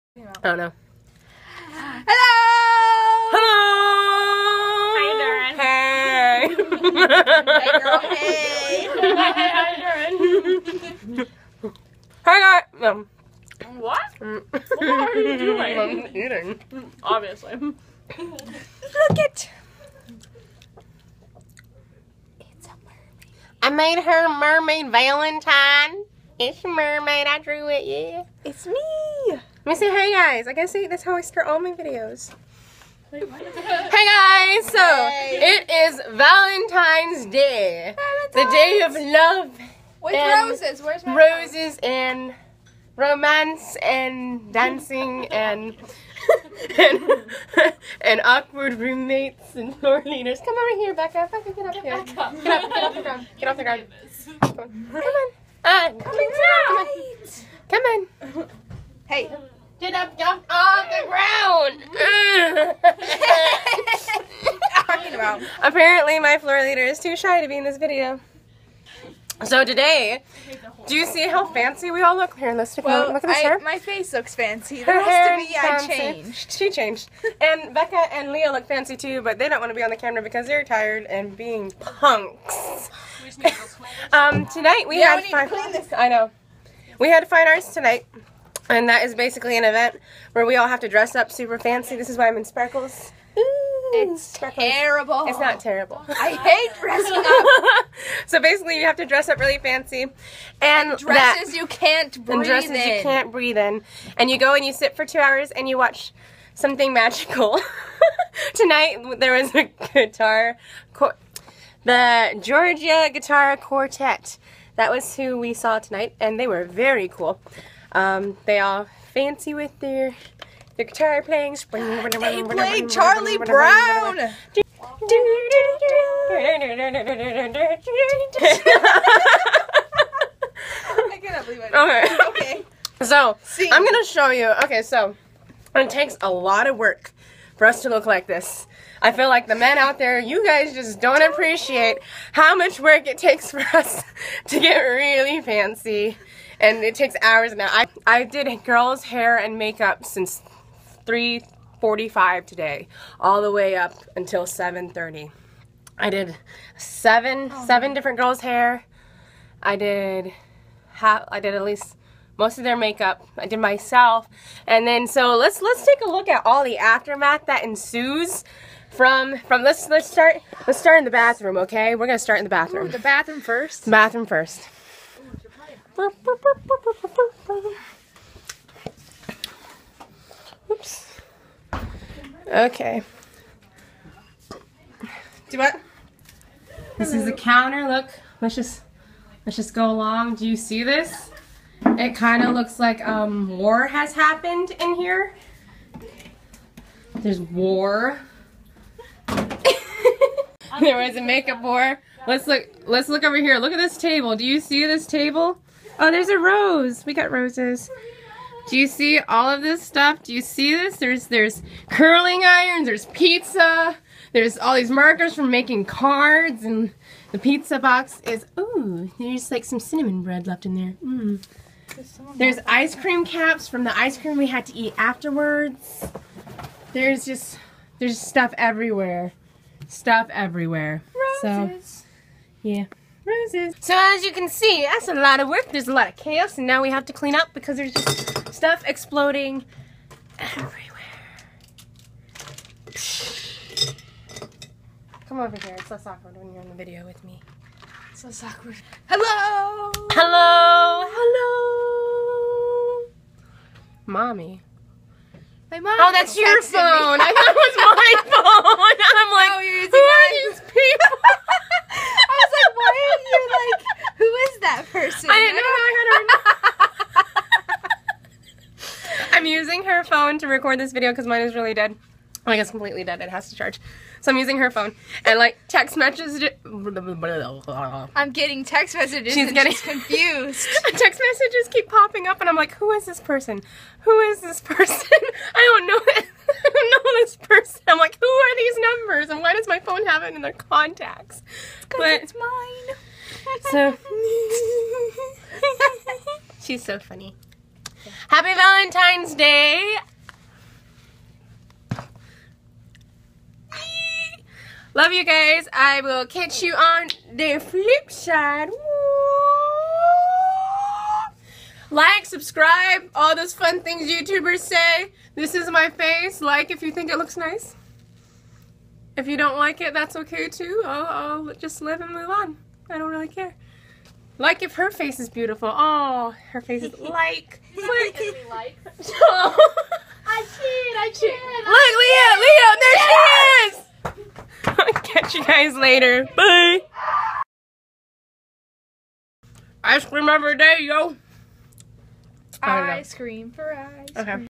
VALENTINES DAY AFTERMATH! Vlog#14
It's Valentines Day, and love is in the air! and by love I mean I went to a thing called Fine Arts and then recorded myself eating icecream...HOLLA! hahahXD **SHOUT OUT to my crazy floor leader Becca! she is a cool cat:)